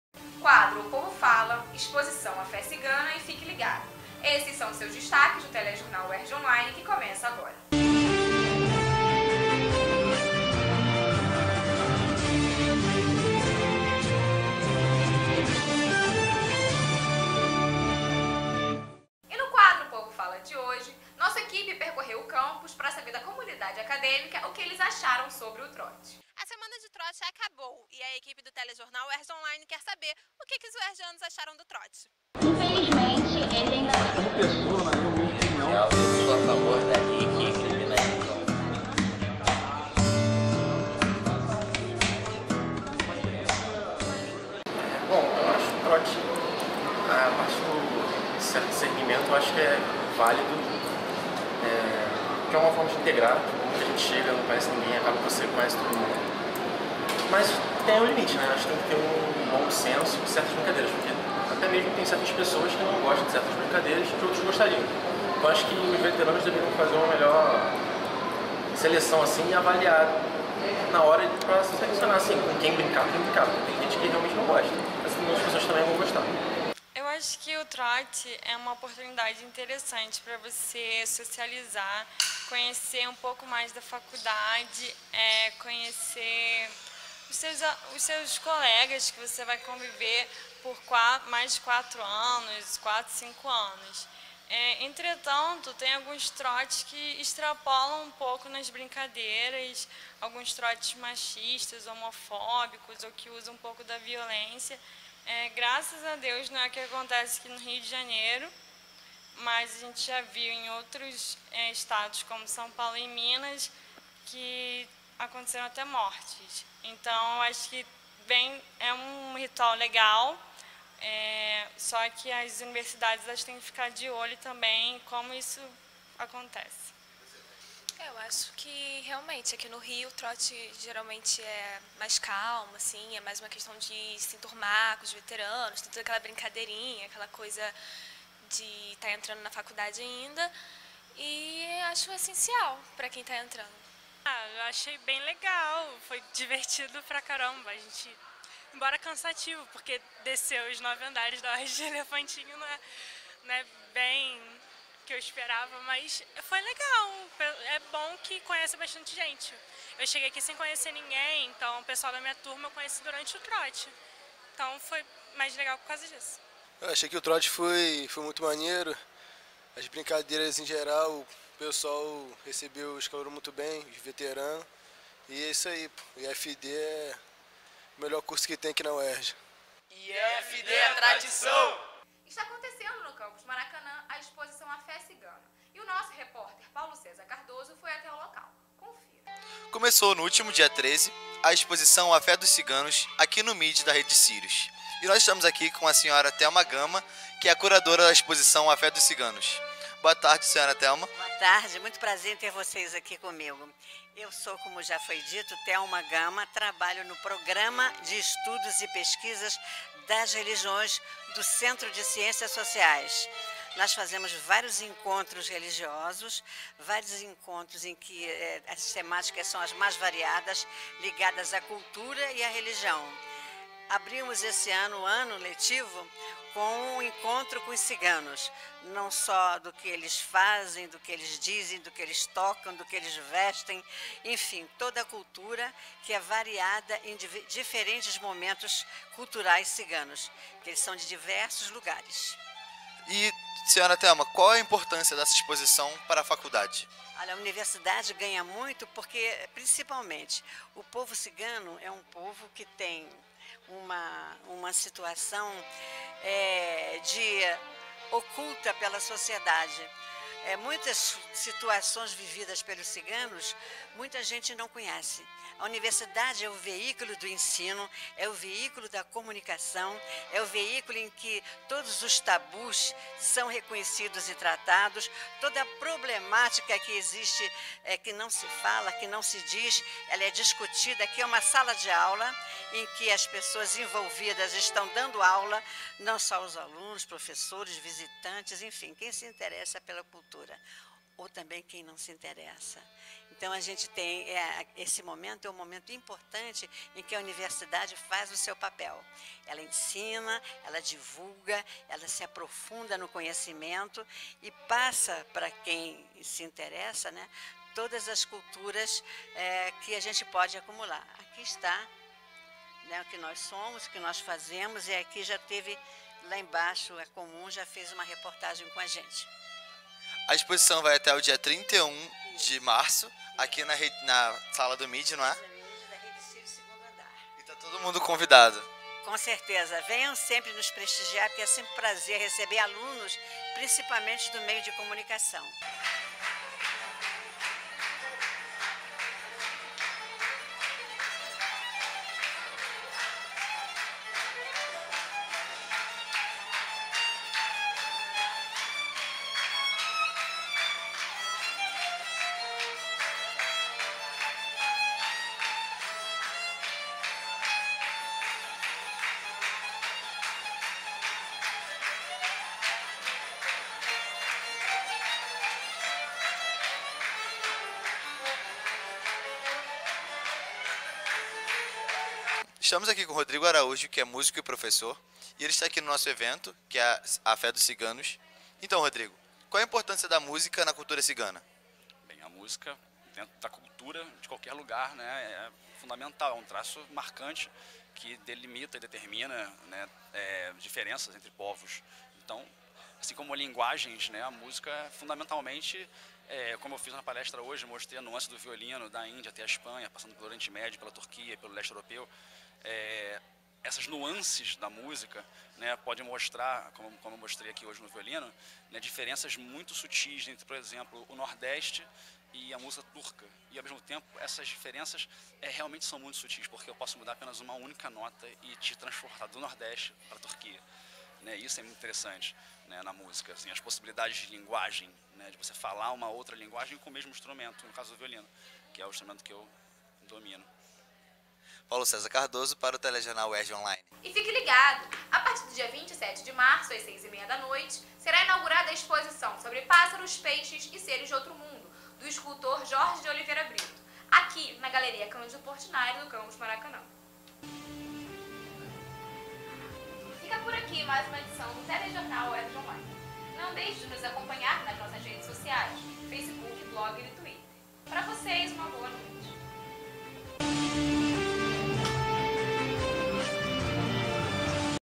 25/03/2014
Nessa edição: Quadro O Povo Fala, Evento A Fé dos Ciganos e Fique Ligado.